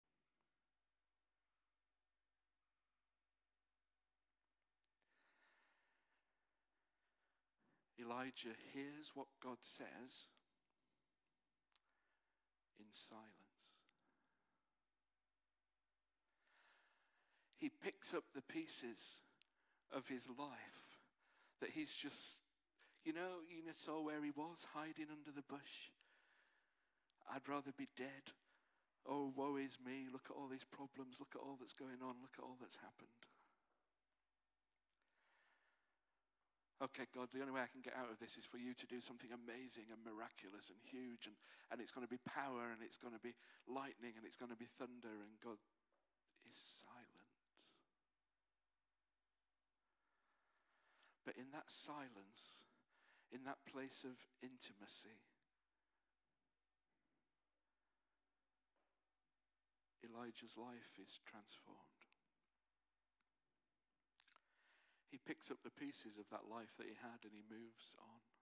Finding God in Silence: Elijah's Transformative Journey
Explore how Elijah's moment of despair leads to divine transformation. Witness the power of silence and intimacy with God that changes his life, as he picks up the pieces and moves forward. Discover the profound lessons embedded in his story.